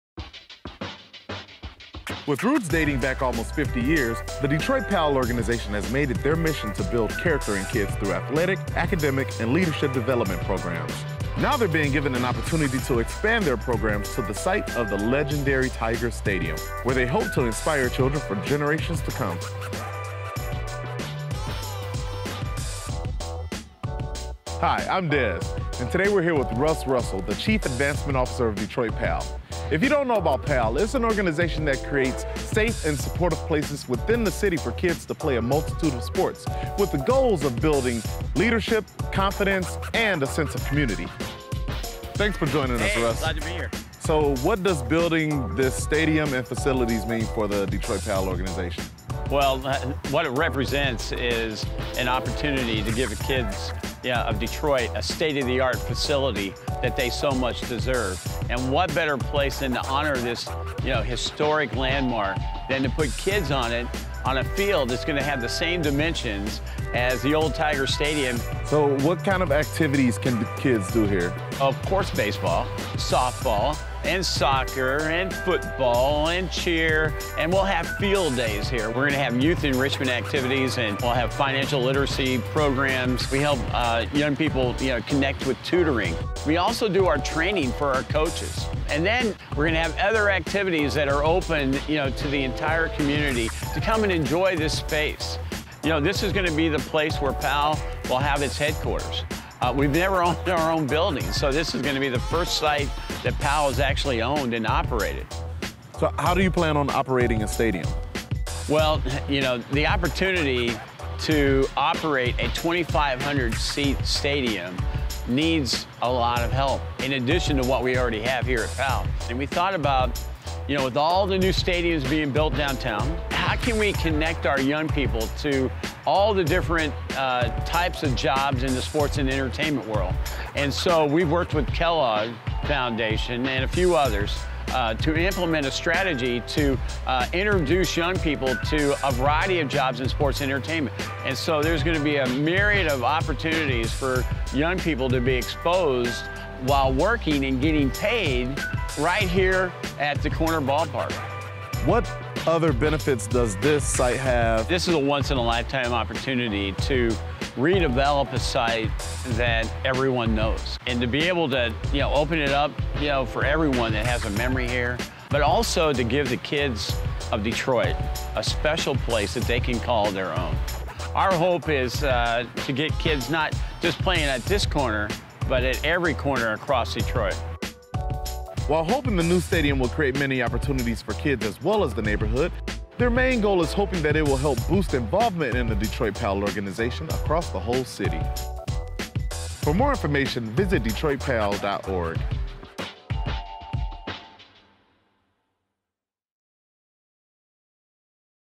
Detroit PAL - Chevy Detroit Champions
Detroit PAL is turing the ground of the old Tiger Stadium into a new stadium for the youth of Detroit.